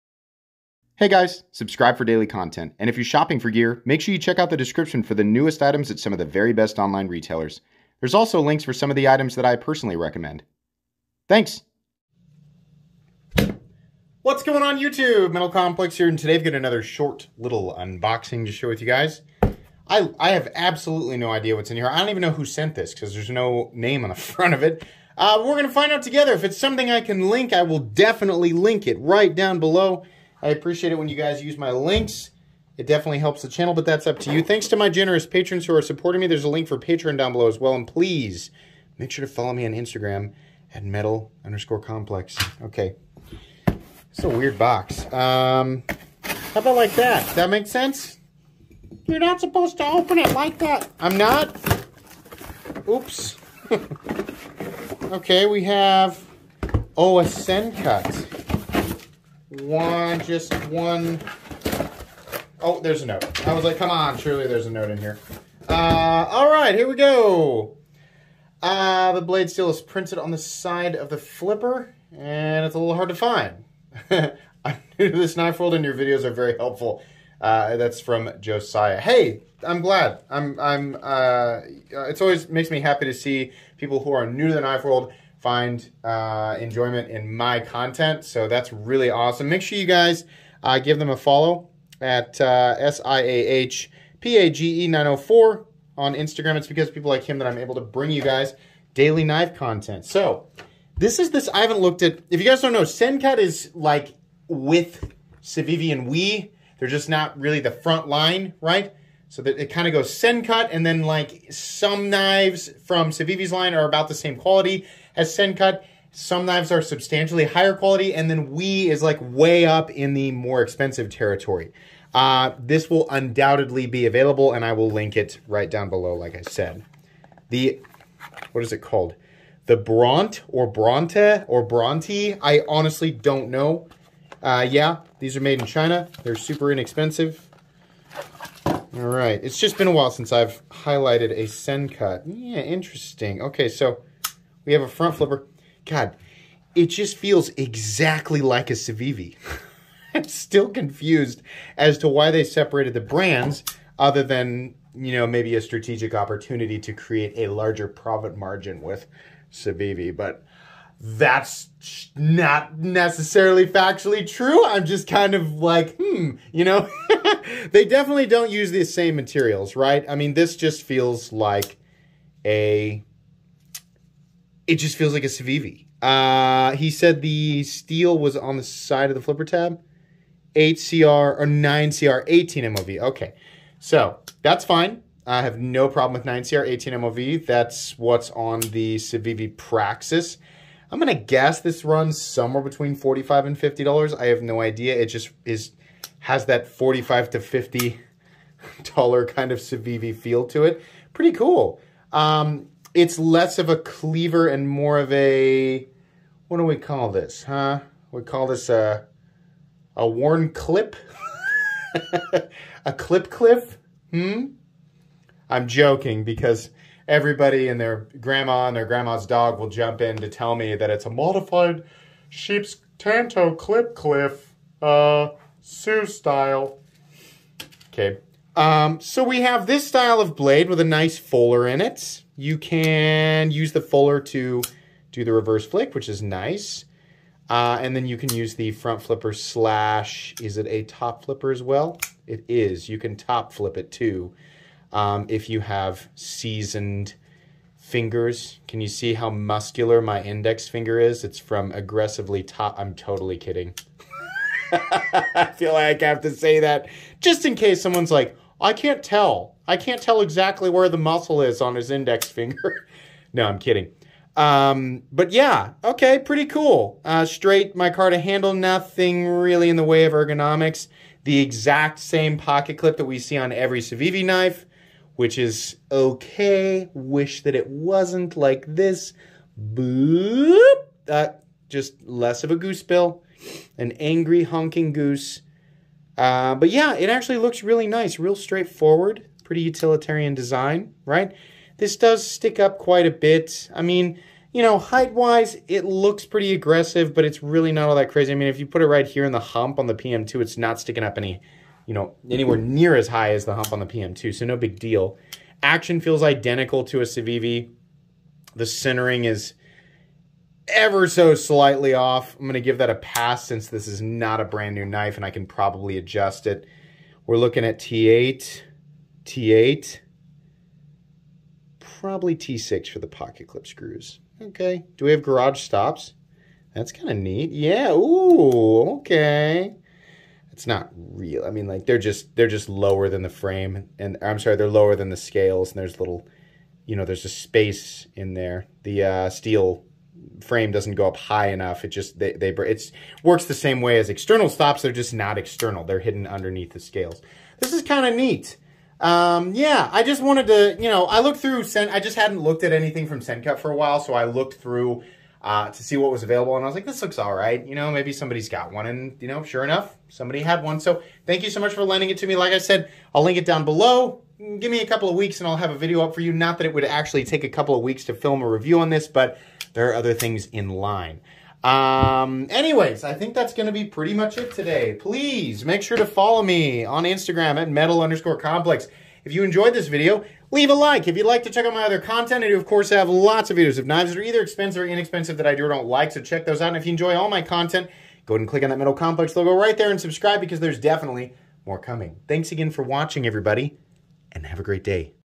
And You Can Buy It Now? - Knife Unboxing
Unboxing the Sencut Bronte.

Sencut Bronte

The tools, lubricants, flashlights, sharpening equipment, and knife storage I use right here on my channel!